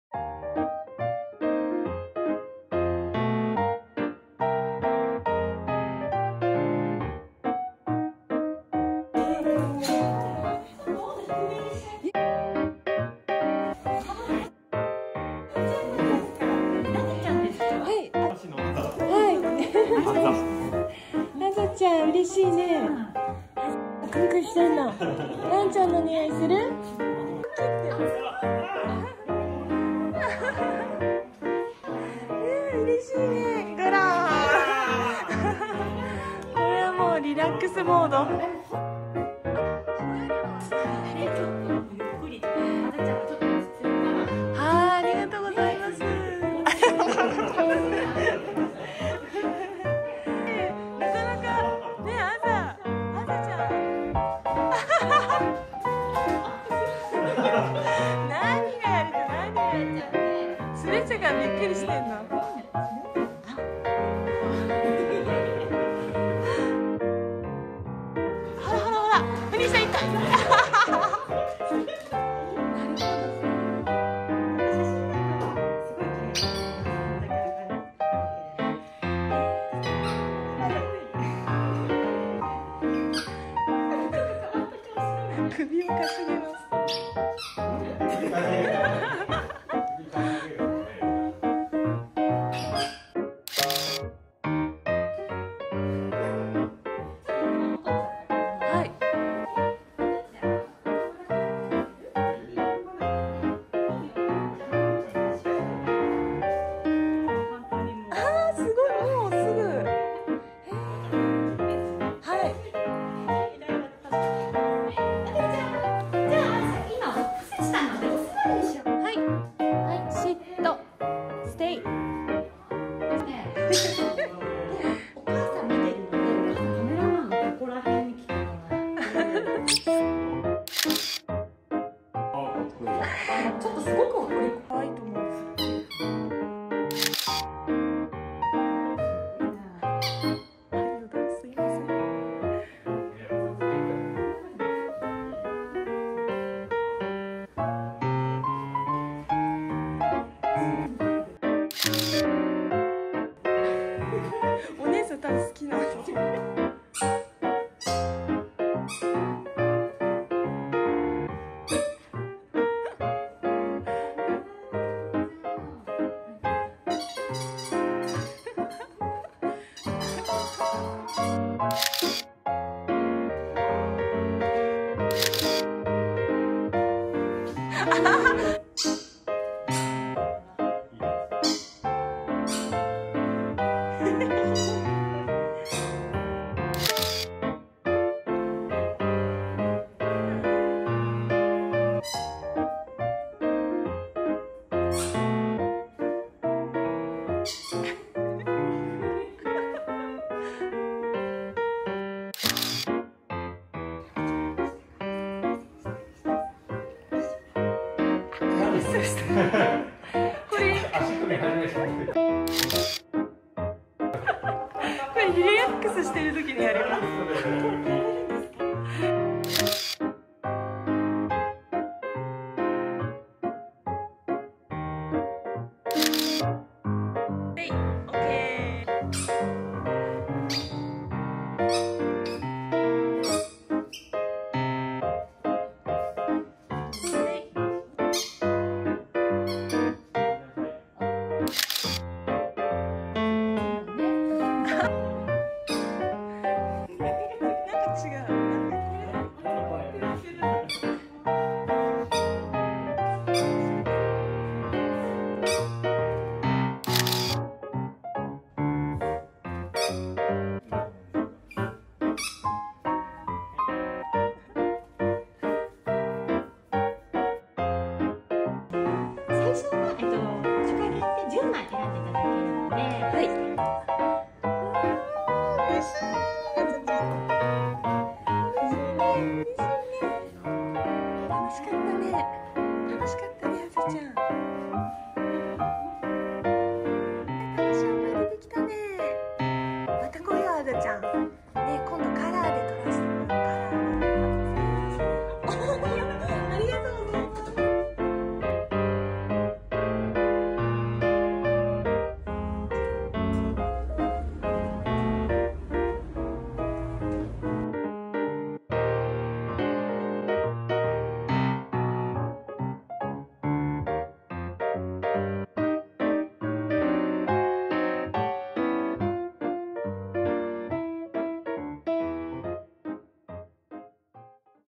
プロのカメラマンに撮影してもらったバーニーズマウンテンドッグ
初めてプロの方に撮影をお願いしました。
とってもキレイに撮っていただき感動。和やかな雰囲気で楽しい時間でした。

▼目次
1:34 1枚目
2:11 2枚目
2:17 3枚目
2:32 4枚目
2:52 5枚目
2:55 6枚目
3:07 7枚目
3:22 8枚目
3:33 9枚目
3:40 10枚目
3:46 11枚目
4:02 12枚目
4:17 13枚目

▼撮影場所
犬・猫専門フォトスタジオ and site
東京都新宿区西新宿3-9-1 西新宿ティーオービル 7階